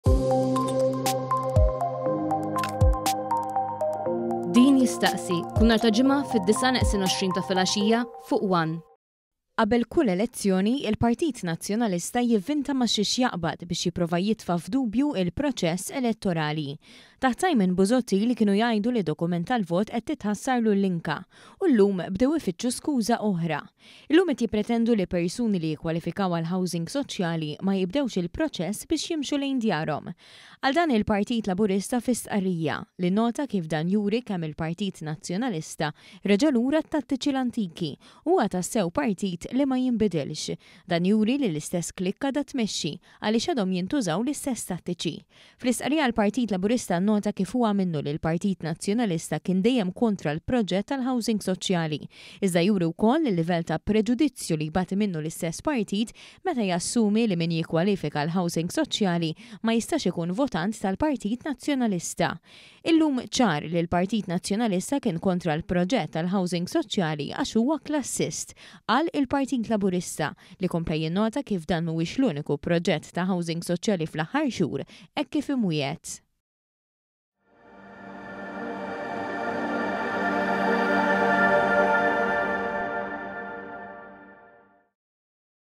IL-PN LURA GĦAT-TATTIĊI L-ANTIKI TA’ QABEL L-ELEZZJONI – IL-PL
ONE.COM.MT •
Qabel kull elezzjoni, il-Partit Nazzjonalista jivvinta ma’ xiex jaqbad biex jipprova jitfa’ f’dubju l-proċess elettorali.

Taħt Simon Busuttil kienu jgħidu li d-dokument tal-vot qed titħassarlu l-linka, u llum bdew ifittxu skuża oħra.

Illum qed jippretendu li persuni li jikkwalifikaw għall-housing soċjali, ma jibdewx il-proċess biex jimxu lejn djarhom.

Qal dan il-Partit Laburista fi stqarrija, li nnota kif dan juri kemm il-Partit Nazzjonalista reġa’ lura għat-tattiċi l-antiki, u huwa tassew partit li ma jinbidilx.

Dan juri li l-istess klikka għadha tmexxi, għax għadhom jintużaw l-istess tattiċi.

Fl-istqarrija, il-Partit Laburista nnota kif huwa minnu li l-Partit Nazzjonalista kien dejjem kontra l-proġett tal-housing soċjali, iżda juri wkoll il-livell ta’ preġudizzju li jbati minnu l-istess partit meta jassumi li min jikkwalifika għall-housing soċjali, ma jistax ikun votant tal-Partit Nazzjonalista.

‘Illum ċar li l-Partit Nazzjonalista kien kontra l-proġett tal-housing soċjali għax huwa klassist’, qal il-Partit Laburista, li kompla jinnota kif dan mhuwiex l-uniku proġett ta’ housing soċjali fl-aħħar xhur, hekk kif imwiegħed.